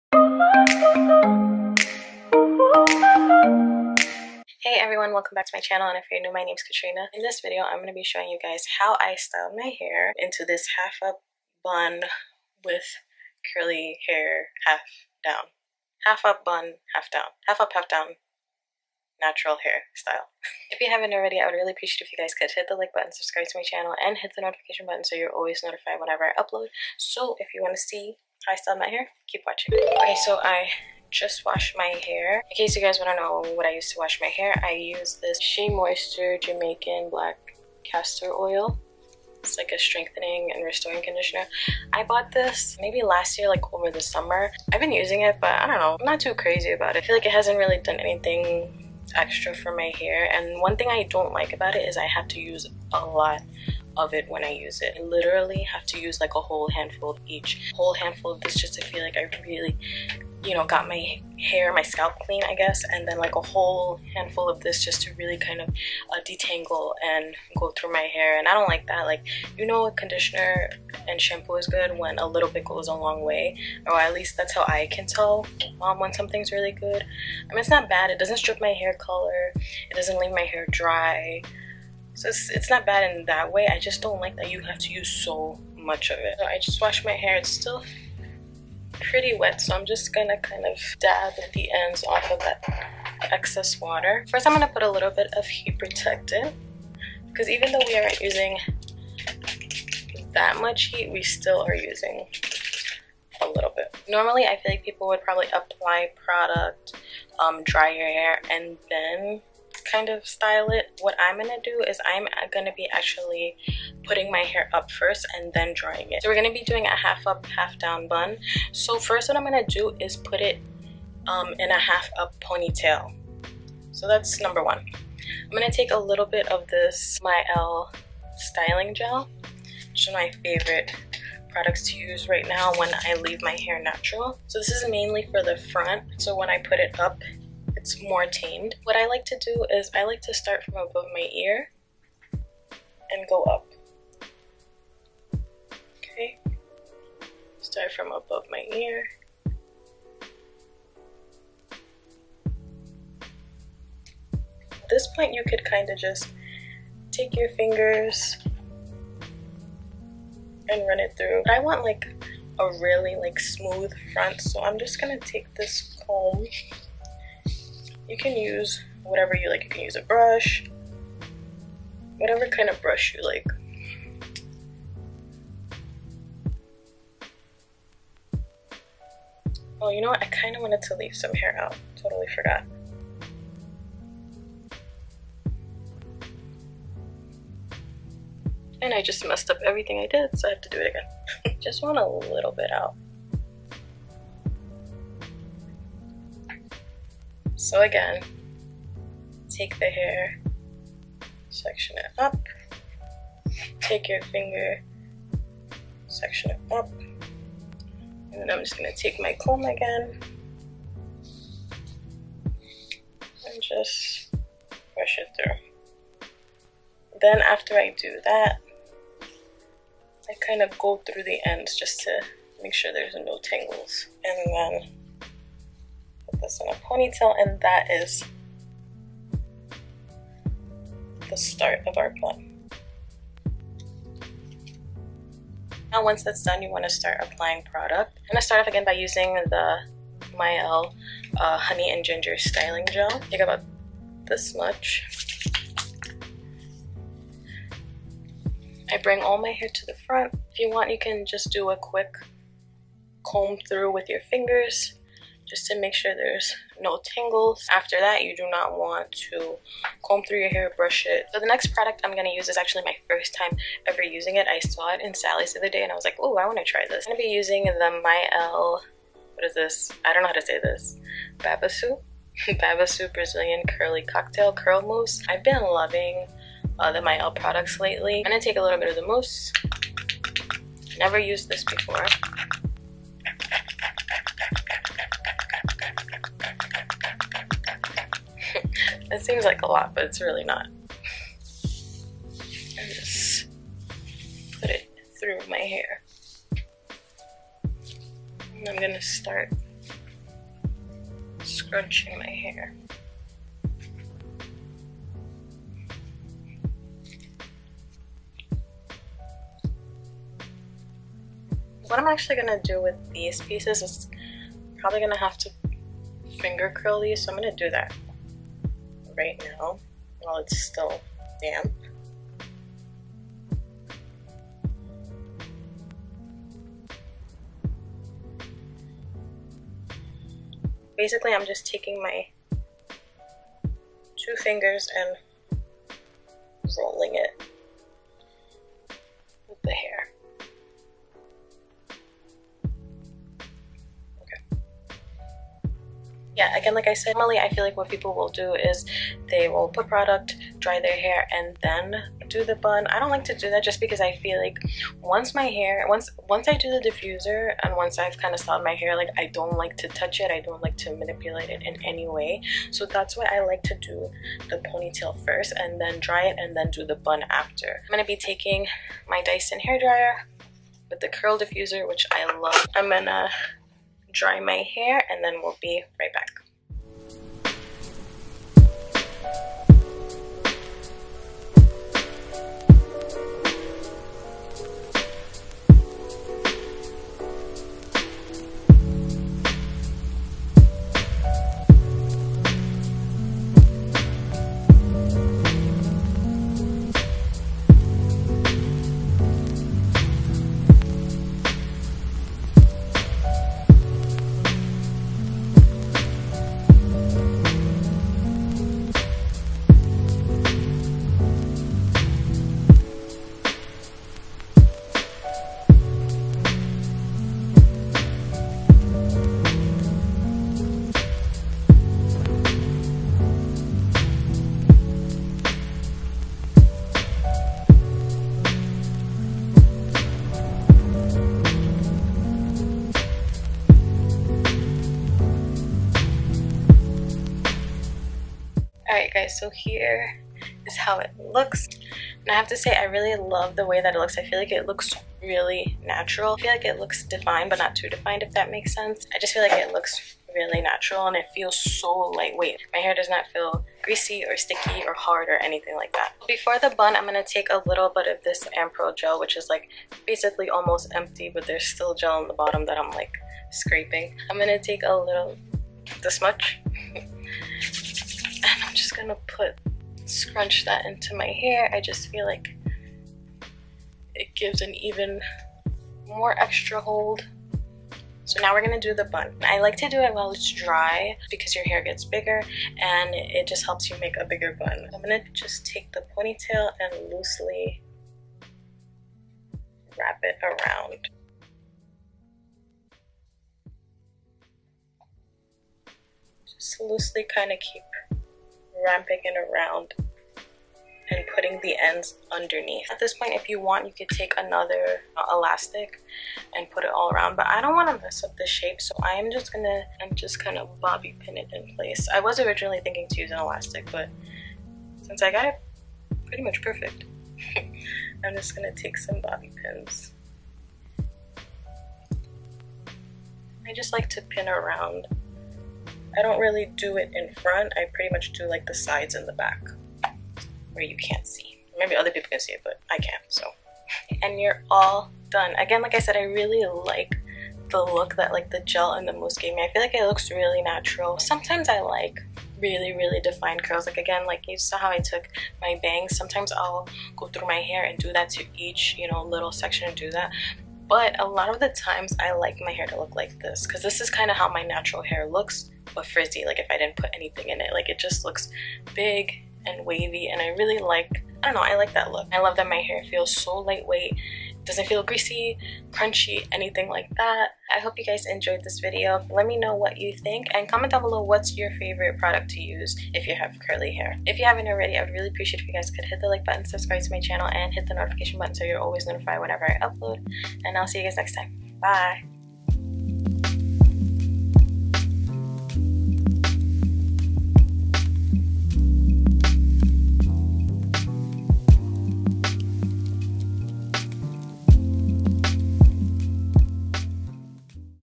Curly Half Up Half Down Hairstyle
In this video I show you how I style my Curly Half Up Half Down Hairstyle.